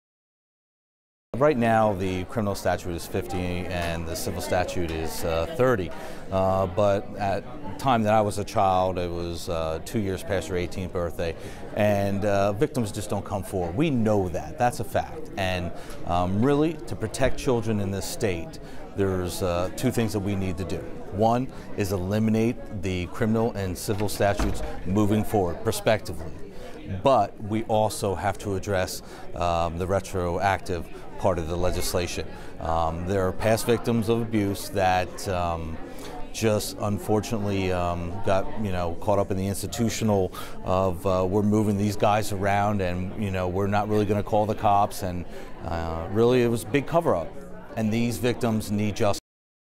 Rozzi: Protect Pa. Kids from Abuse
Pa. state Rep. Mark Rozzi says current law doesn't go far enough in providing a voice for those who have been sexually abused. Many times, child sexual abuse survivors wait decades to share their stories, but it's too late to hold their abusers accountable. Rep. Rozzi's House Bill 2067 would give these survivors more time to press charges against those who have wronged them. To see a clip of a documentary Rozzi commissioned about the urgent need for this legislation,